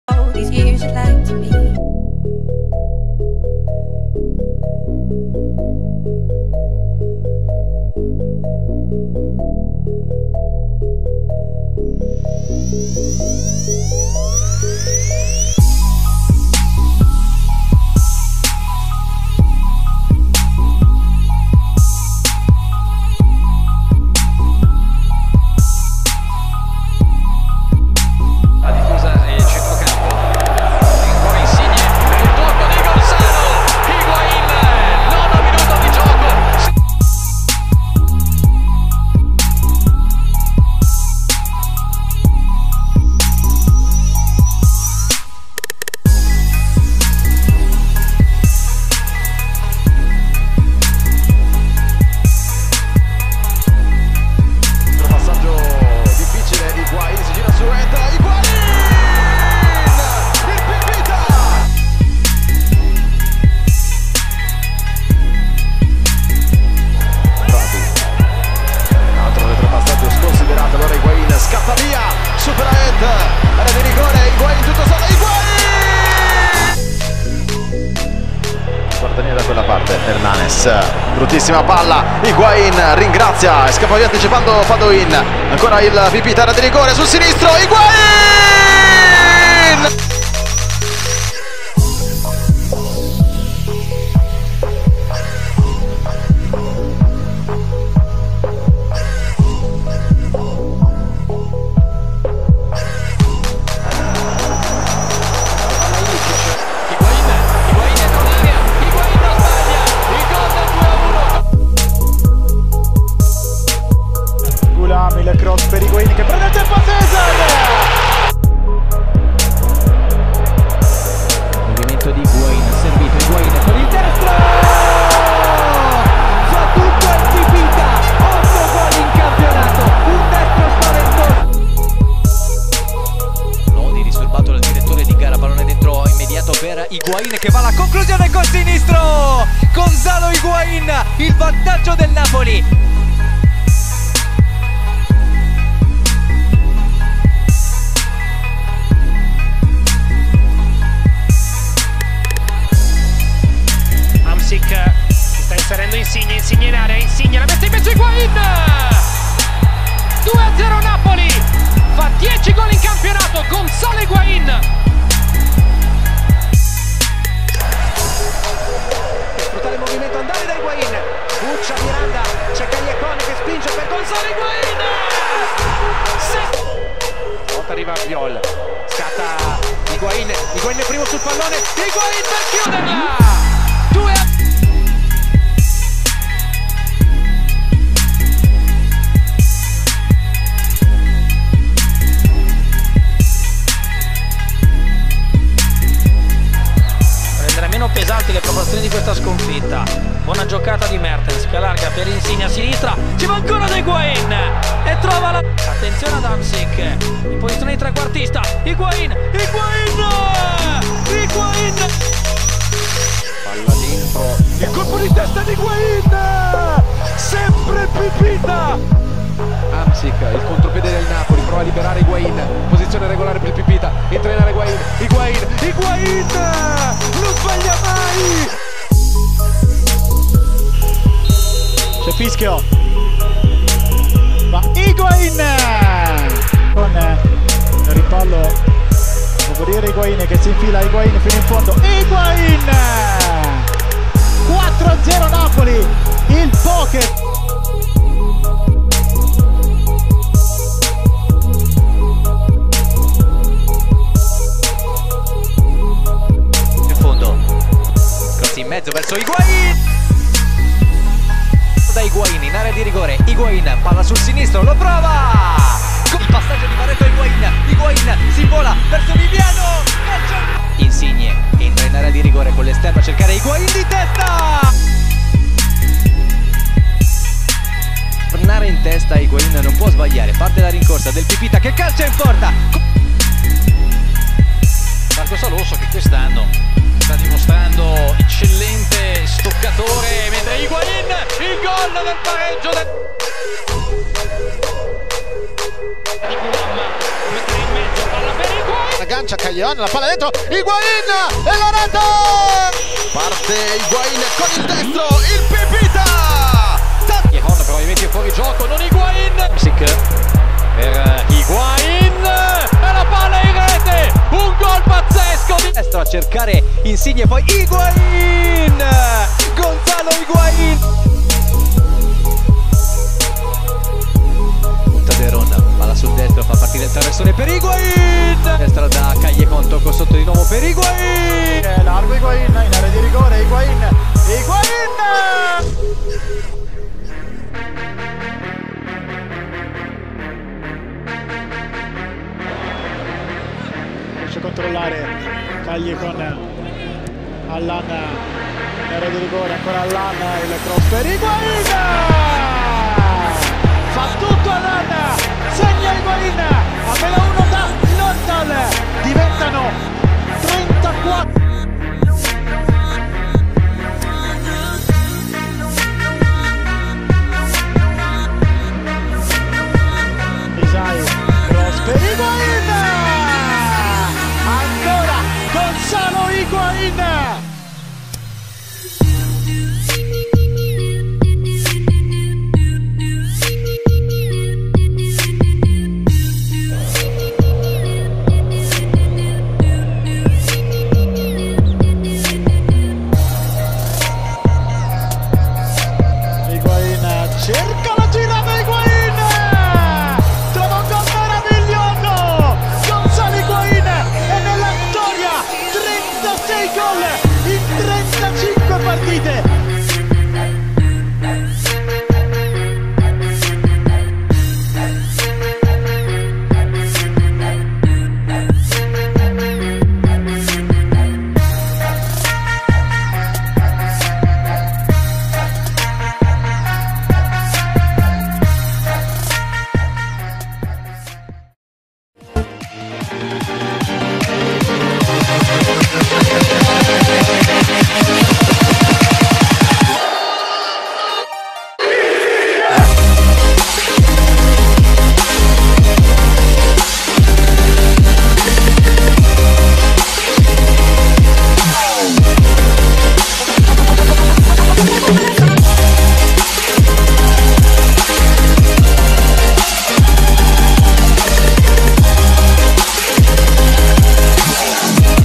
GONZALO HIGUAIN | All 36 Goals 2015/16 | HD
Gonzalo Higuain has probably had the best season of his life. I wanted to share this emotional journey with you guys. There has been ups and downs for SSC Napoli and Gonzalo Higuain the big number 9. Higuain broke the record for the most goals scored in one full season. He was on 33 goals (record was 35) at the last game. To beat it he had to score a hat-trick and so he did by breaking the record with a bicycle kick. Truly incredible. Napoli broke the record of the most points they had ever got in one season and were crowned winter champions. What a season and may the next one be even better